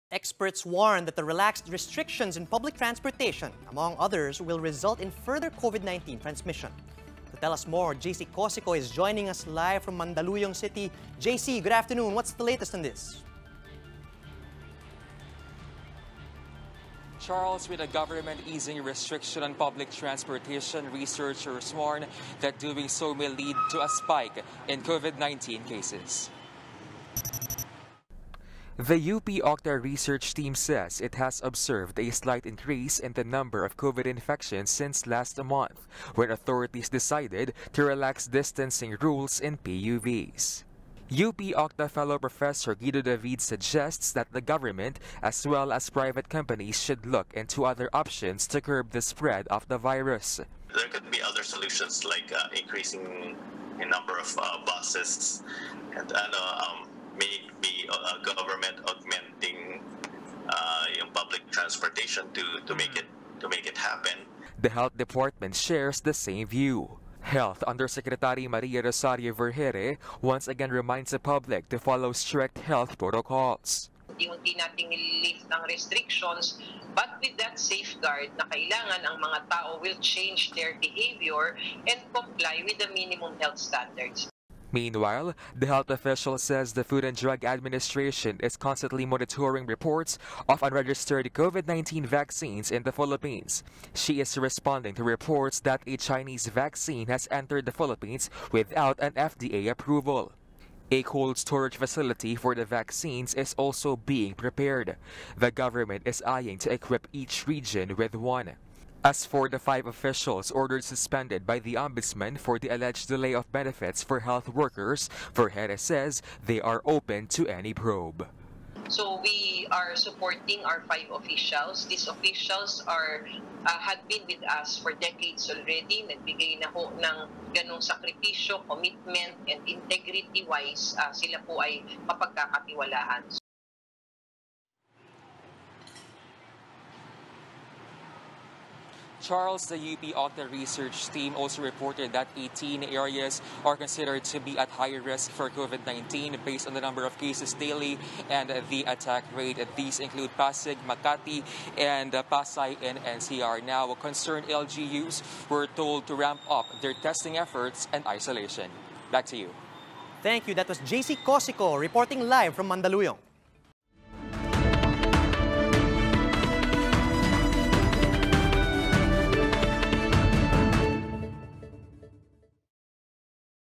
Gov't eyes cold storage facility in every region for COVID-19 vaccine
DOH assures FDA constantly monitoring reports of COVID-19 vaccines in the Philippines, following alleged entry of unregistered Chinese vaccine in the country.